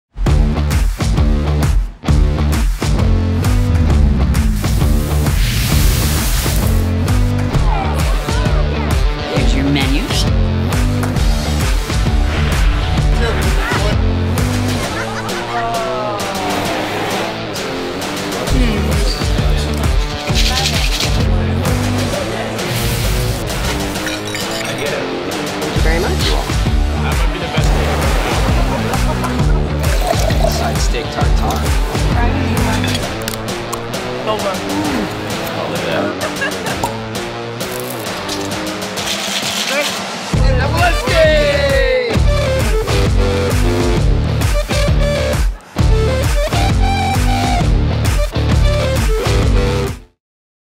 The Smith & Wollensky Experience
See the Smith & Wollensky experience for yourself. USDA Prime dry-aged steaks, premium seafood, award-winning wines, hand-crafted cocktails and impeccable service.